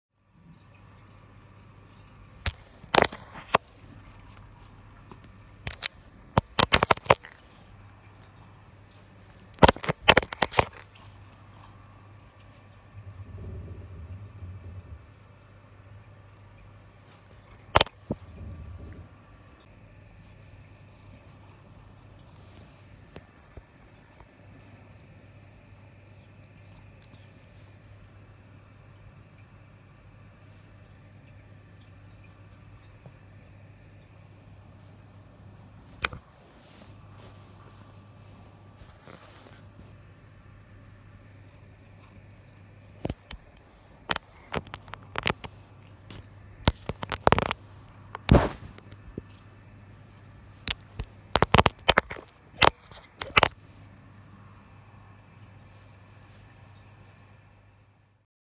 【マンチカンズ】ニャンコに喰われるVR? ~ VR cat bite! ~
マンチカンのルナくんはカメラが大好物？痛いよやめてー。